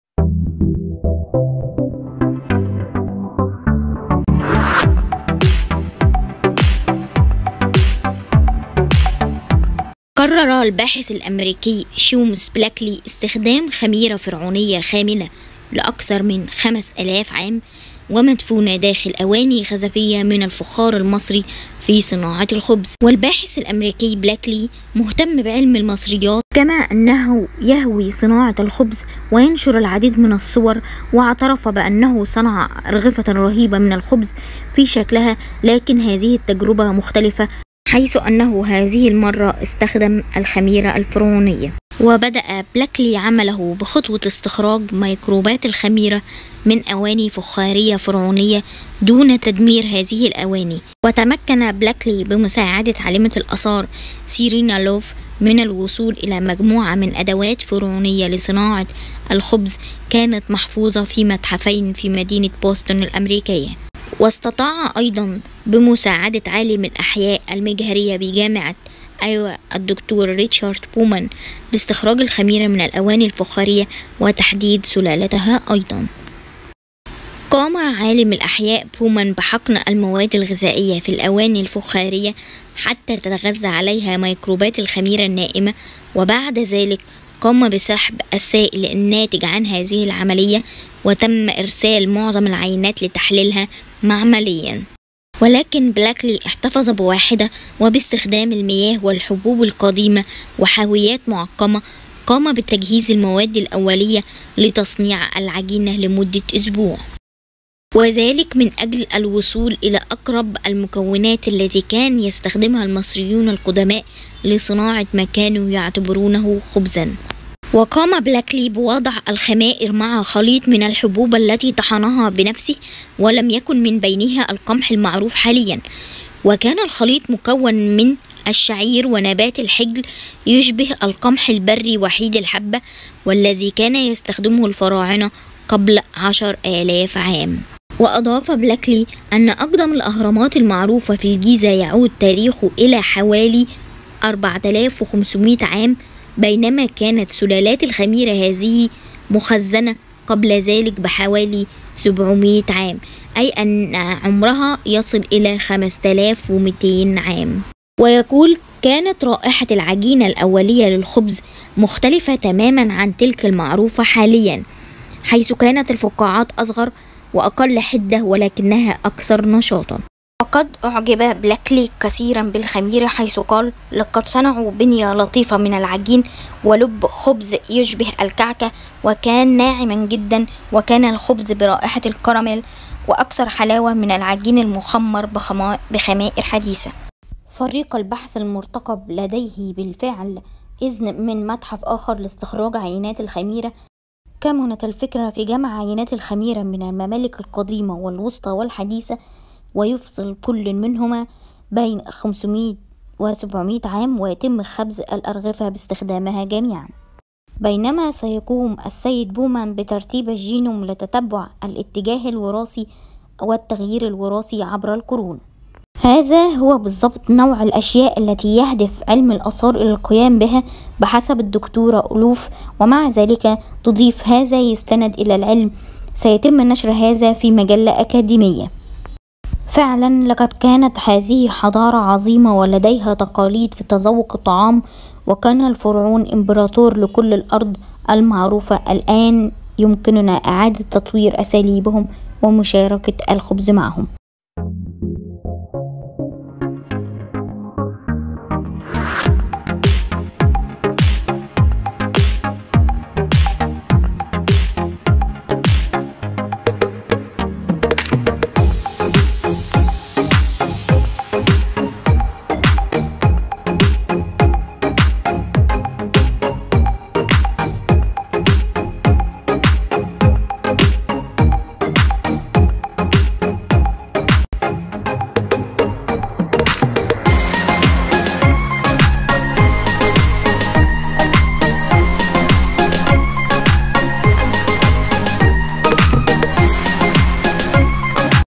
العيش الفرعوني مصنع من خميره فرعونية متجمعه من فخاره اثريه🤩وطعمه حلو
العيش الفرعوني مصنوع من خميره من ايام الفراعنه متجمعه من إناء خبز فرعوني
Pharaohs bread 🍞 made from yeast collected from braking jar 🏺 used by pharaohs